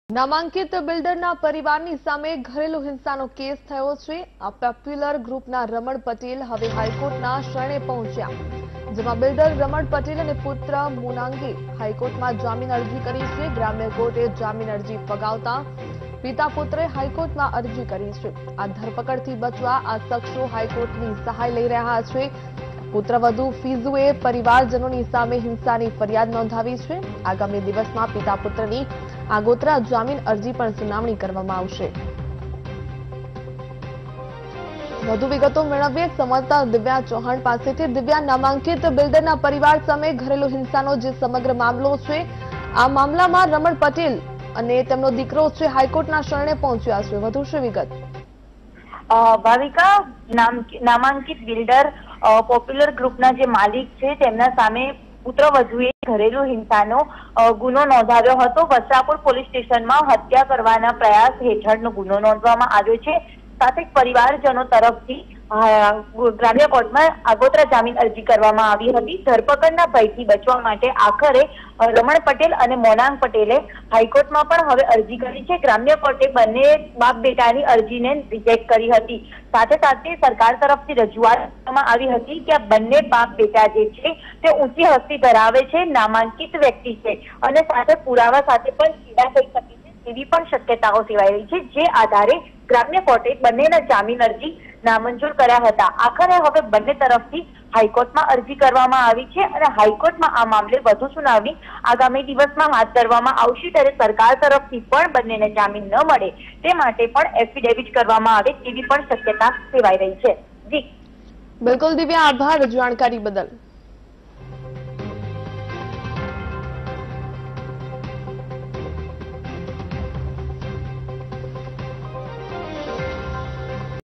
Ahmedabad ના પોપ્યુલર બિલ્ડરનનો કેસઃ આગોતરા જામીન માટે પિતા-પુત્ર હાઈકોર્ટ પહોંચ્યા | VTV Gujarati
Ahmedabad ના પોપ્યુલર બિલ્ડરનનો કેસઃ આગોતરા જામીન માટે પિતા-પુત્ર હાઈકોર્ટ પહોંચ્યા